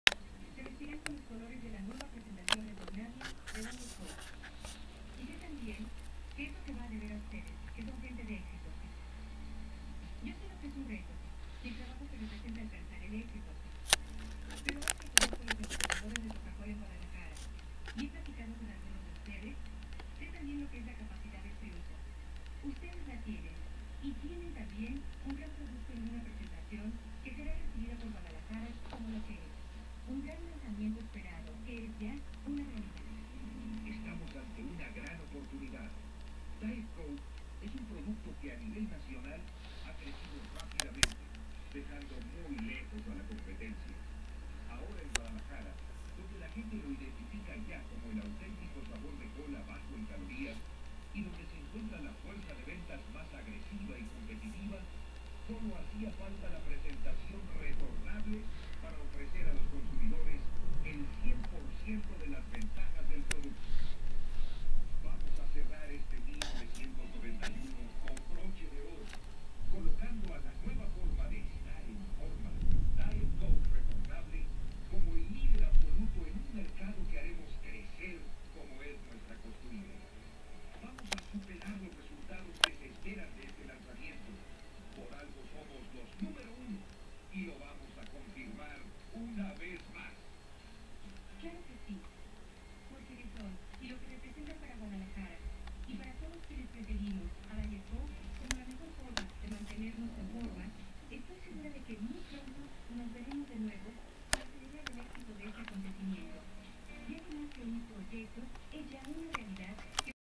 VCR Beta SONY Hi-Fi SL-HF350
Prueba de funcionamiento de una VCR SONY Hi-Fi SL-HF350 en una pantalla de plasma samsung de 50".